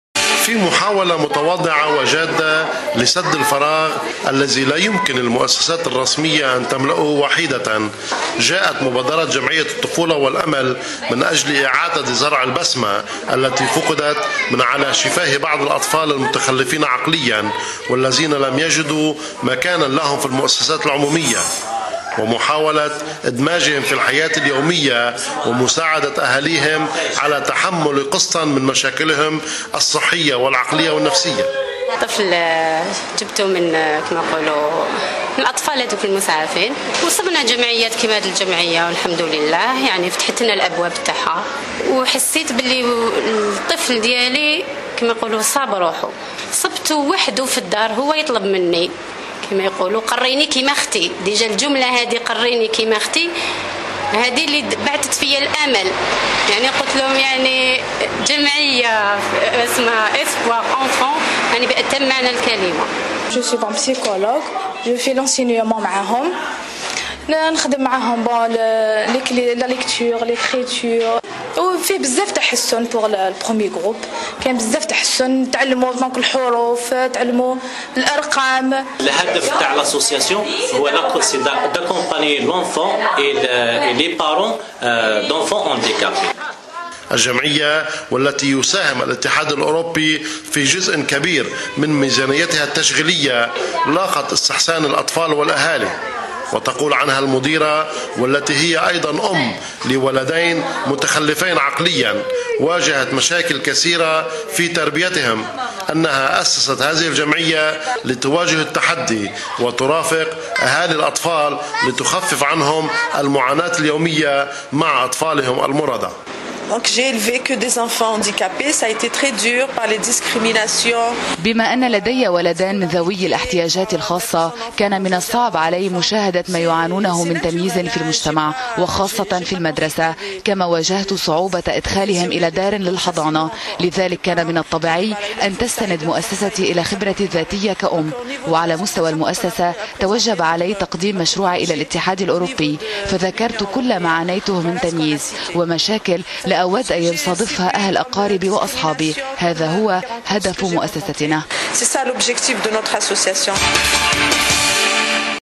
A glimpse of hope for children with special needs (TV report from Eurojar
The European Union provides financial and technical support to the Hope for Children Algerian association, which aims not only at helping children with special needs, but also at accompanying their disorientated parents. A live report on how to overcome disabilities.